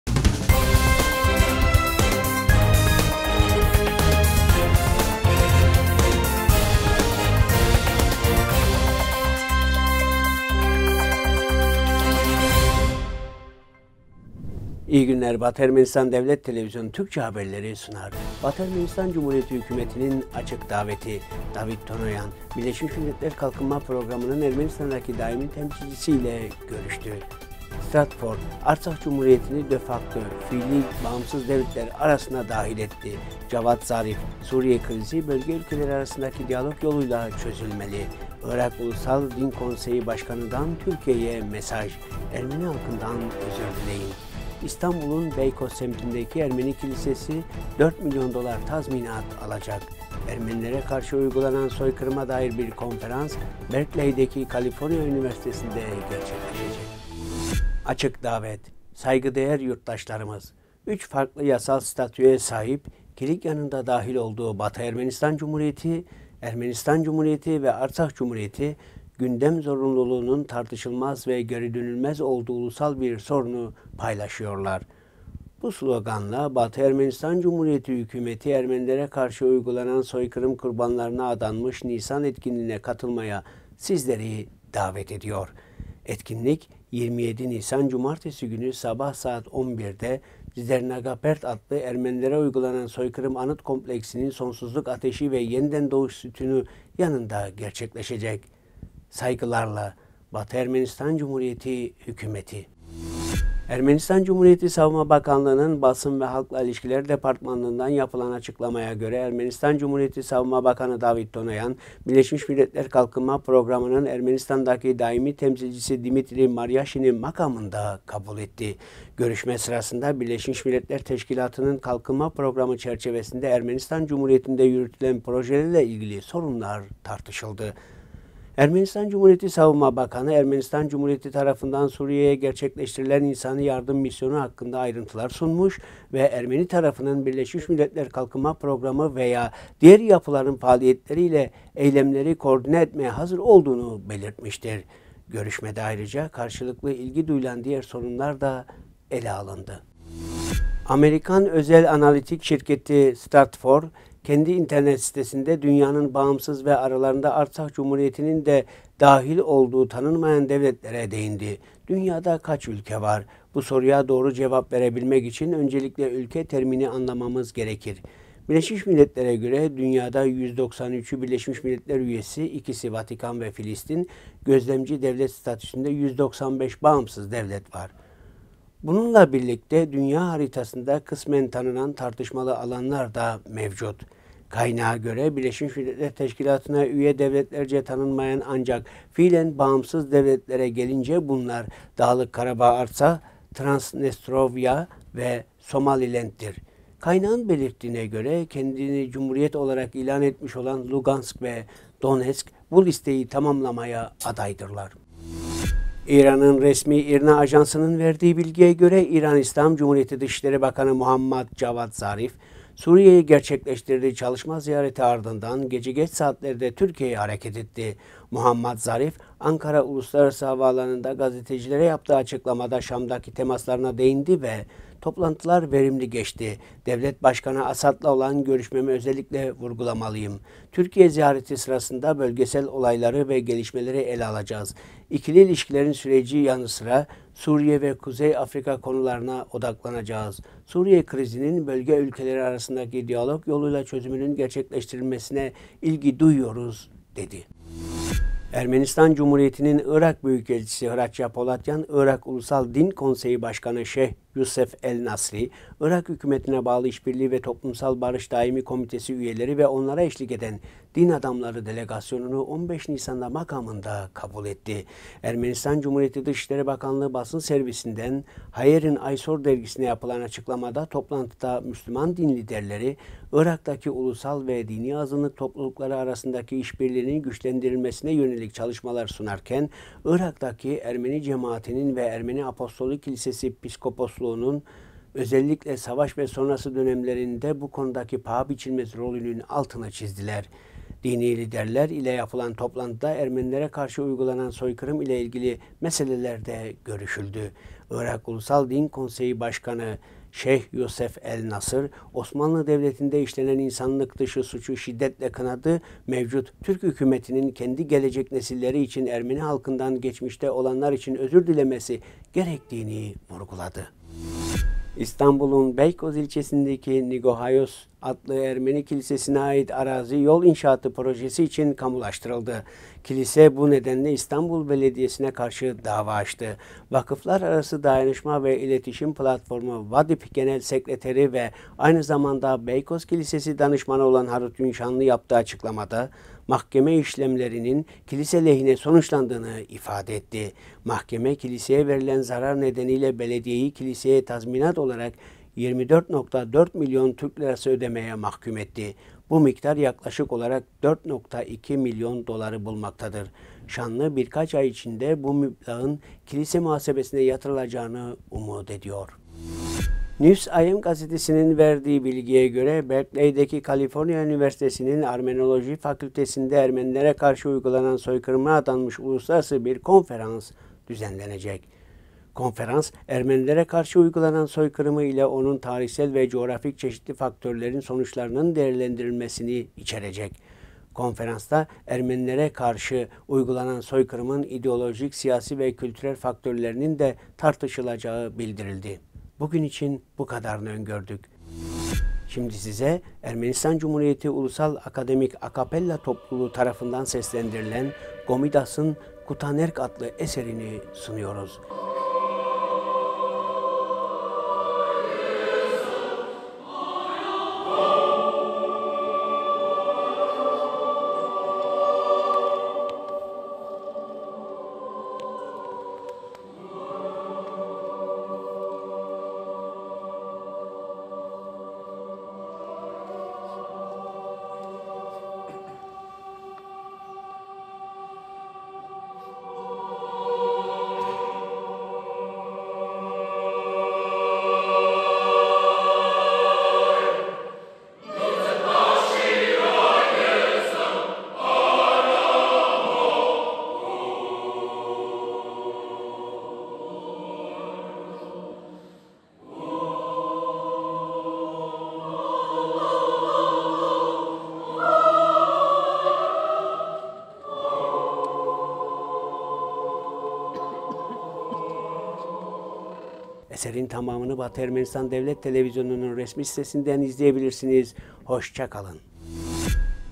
İstanbul’un Beykoz semtindeki Ermeni kilisesi 4 milyon dolar tazminat alacak.Haberler 2019-04-17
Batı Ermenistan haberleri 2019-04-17

1- Batı Ermenistan Cumhuriyeti hükümetinin daveti
2- Davit Tonoyan, Birleşmiş Milletler Kalkınma Programının Ermenistan'daki Daimi Temsilcisi ile görüştü
3- Stratfor Artsakh Cumhuriyet’ini dö fakto fiili bağımsız devletler arasına dahil etti
4- Cavat Zarif: "Suriye krizi, bölge ülkeleri arasındaki diyalog yoluyla çözülmeli"
5- Irak Ulusal Din Konseyi Başkanı’ndan Türkiye’ye mesaj: Ermeni halkından özür dileyin.
6- İstanbul’un Beykoz semtindeki Ermeni kilisesi 4 milyon dolar tazminat alacak
7- Ermenilere karşı uygulanan soykırıma dair bir konferans,  Berkeley’deki   California Üniversitesi'nde gerçekleşecek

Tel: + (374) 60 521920
Adres: Agatangeghos 2, Erivan Ermenistan